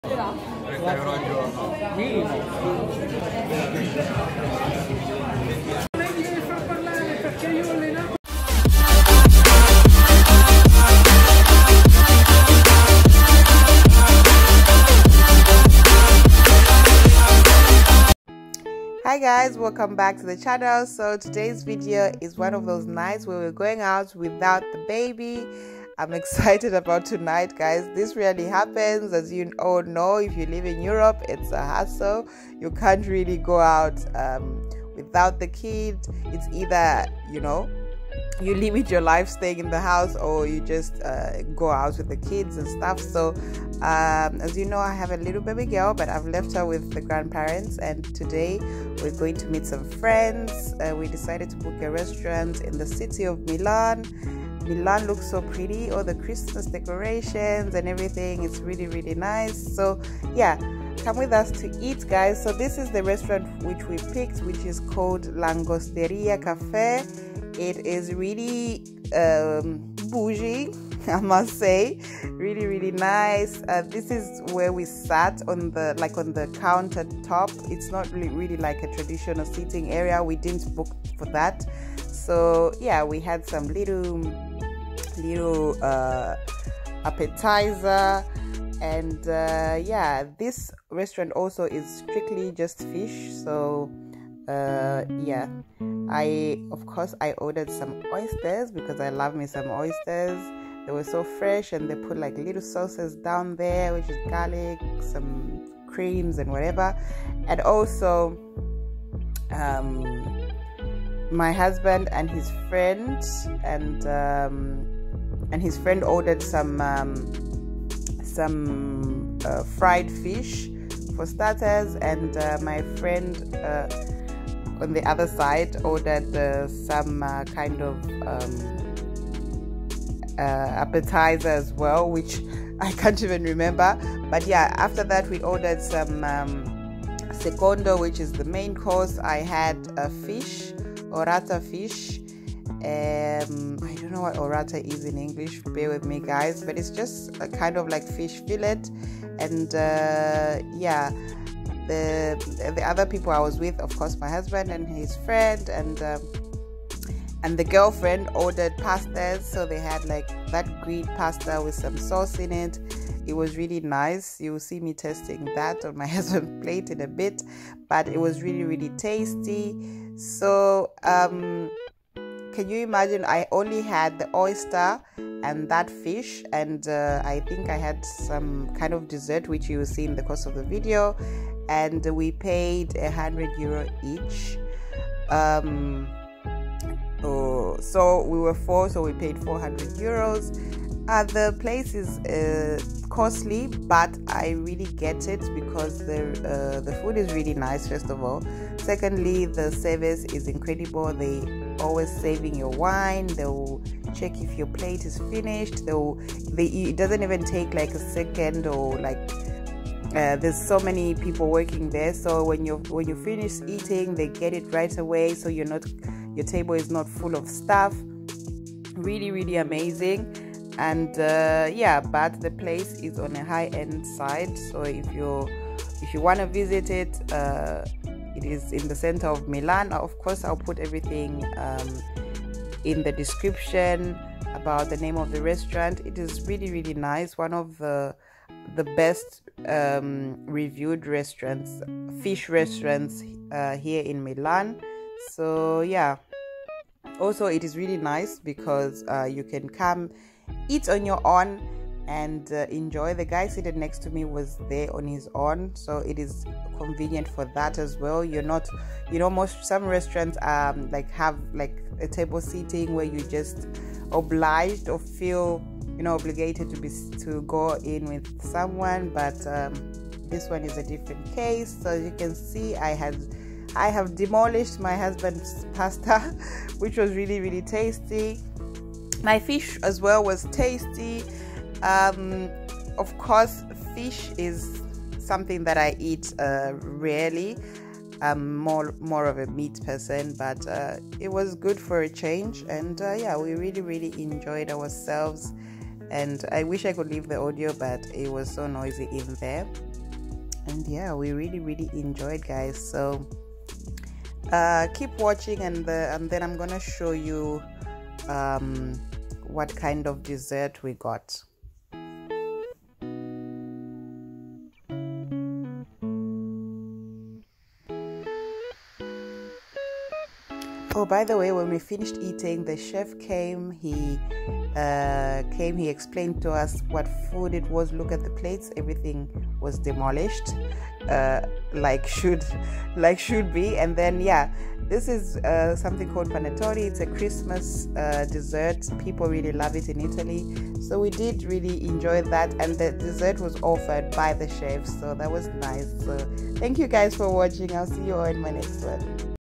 VLOGMAS DAY 15| 400euro for a 2course meal 🍽 worth it?🤔 |Restaurant review
heyii guys here a foodievlog for you all .
restaurant name is called langosteria milan.